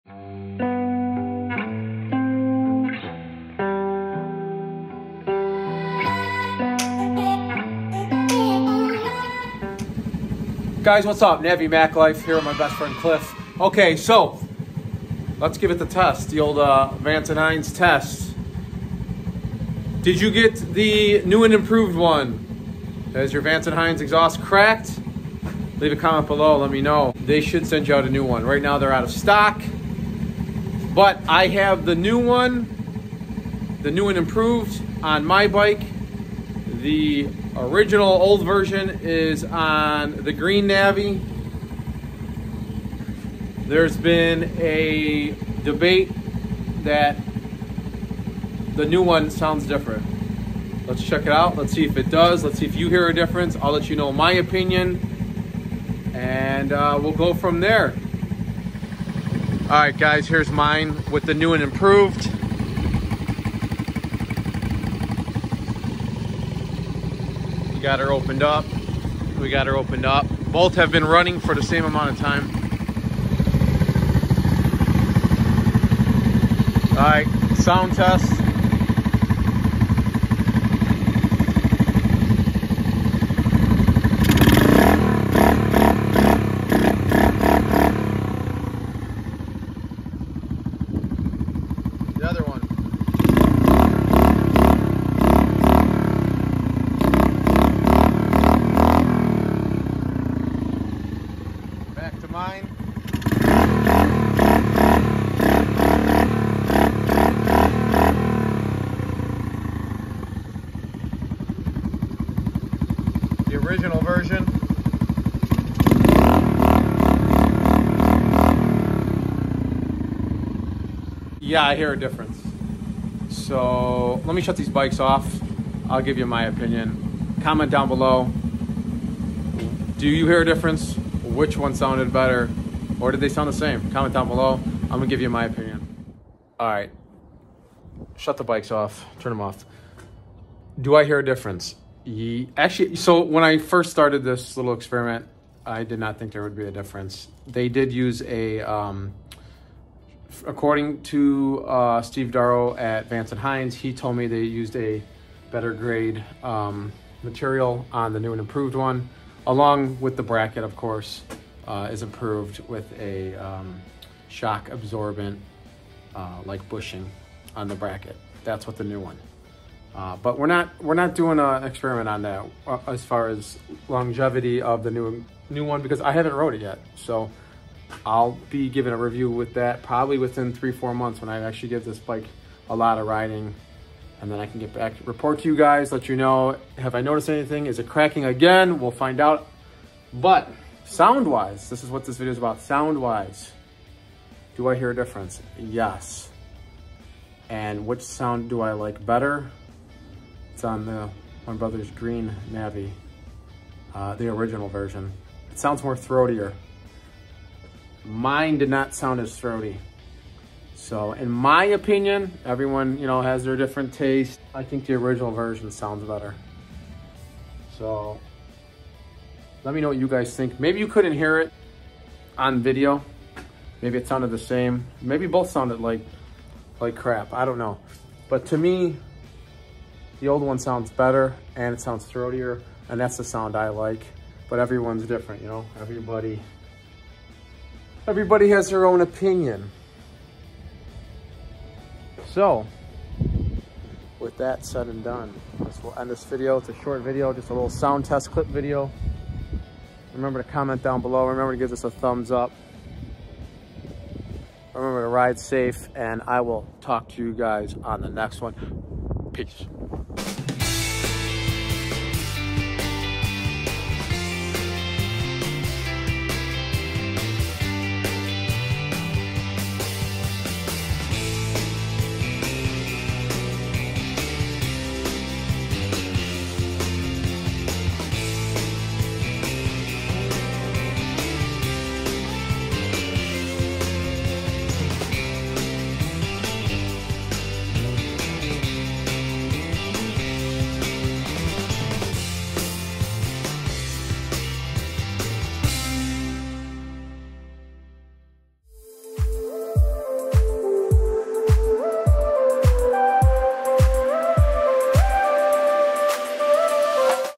Honda Navi/ Comparing sound OLD vs NEW Vance & Hines Exhaust
Which V&H exhaust sounds better? The old one that cracks or the new and improved? Find out!

Released: 2020